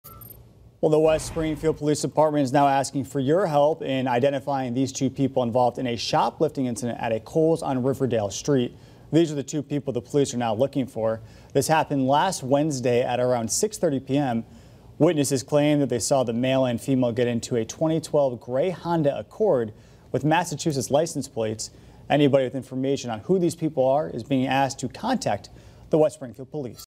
West Springfield Police search for two larceny suspects
Police in West Springfield seek the public’s help in finding two larceny suspects.